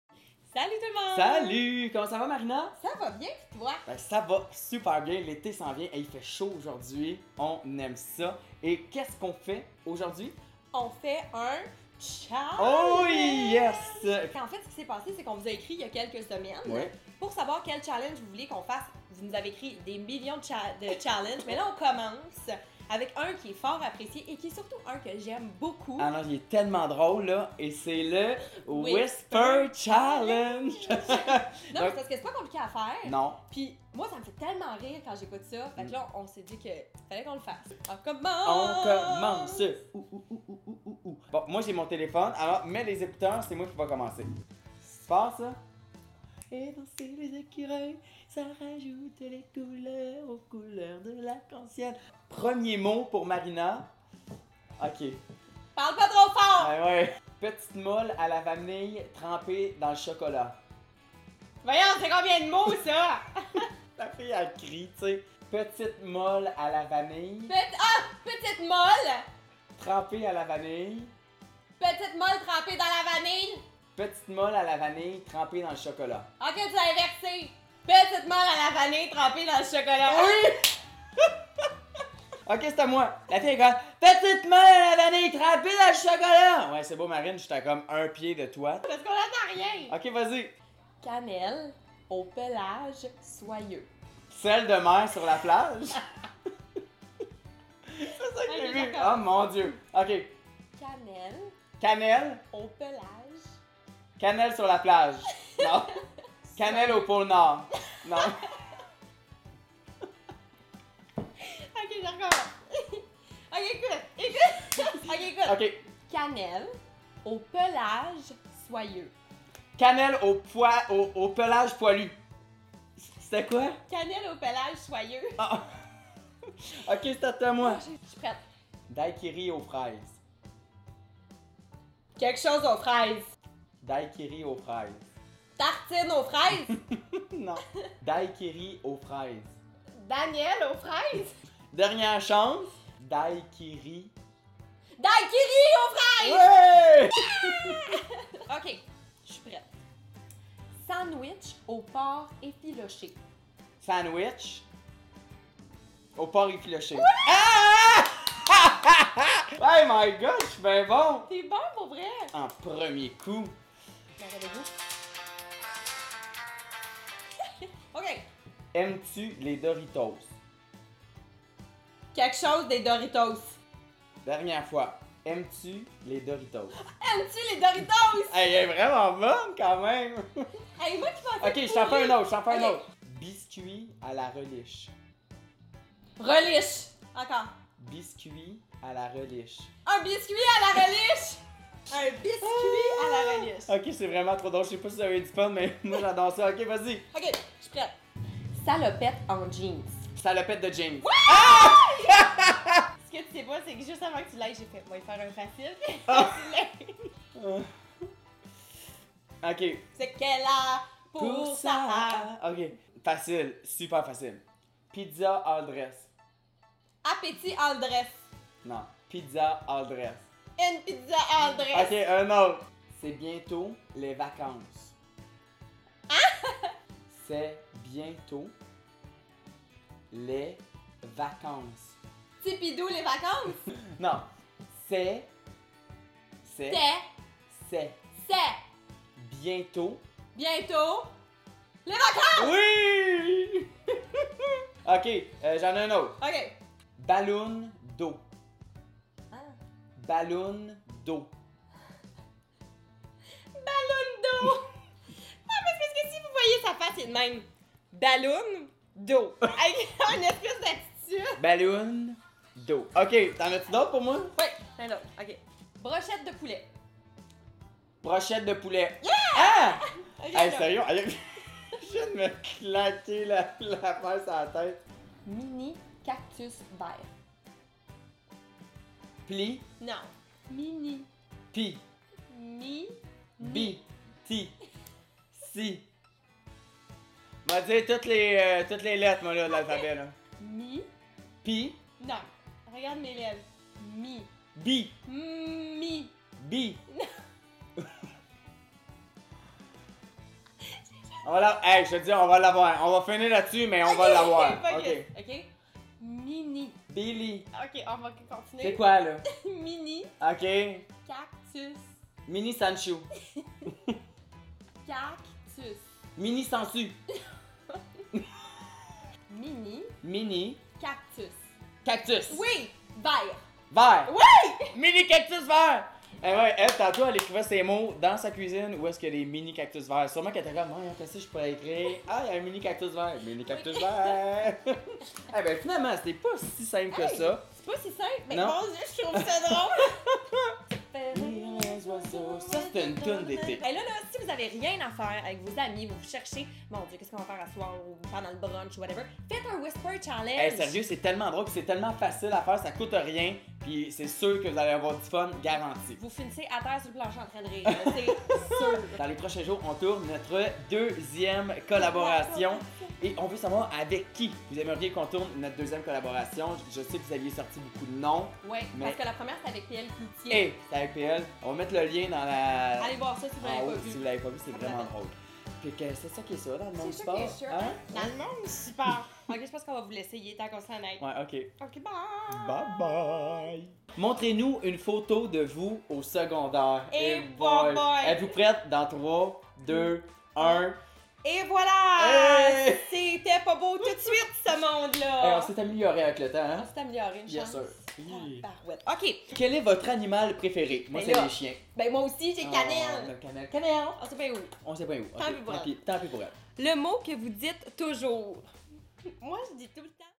Whisper Challenge ( P.O et MARINA)
Vous avez tellement été nombreux à nous écrire vos défis préférés! WOW!  Beaucoup d'entre vous nous ont demandé de faire le JELLY BEAN challenge mais on vous prépare quelque chose d'encore plus drôle!! À suivre! On a décidé d'y aller avec un défi qui faire MOURIR DE RIRE Marina à chaque fois!

On voulait aussi vous dire à quel point on est content de vous lire à chaque semaine! Vous êtes trop hot! On l'apprécie!

RETROUVEZ NOUS!